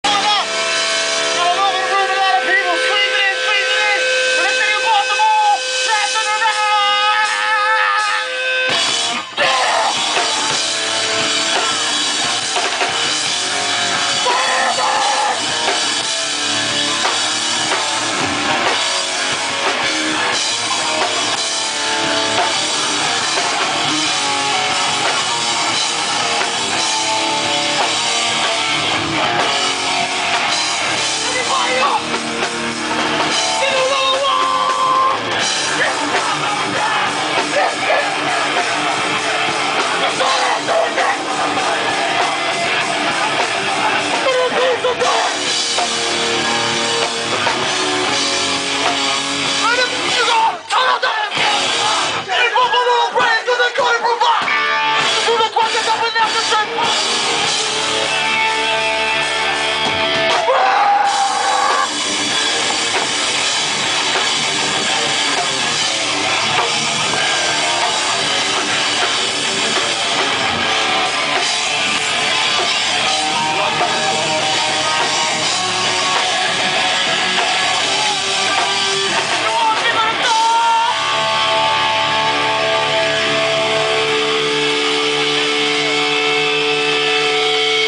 Trapped Under Ice 1 @ Talent Farm 9-6-2010
Trapped Under Ice 1 @ Talent Farm 9-6-2010 w/ bane, cruel hand, alpha and omega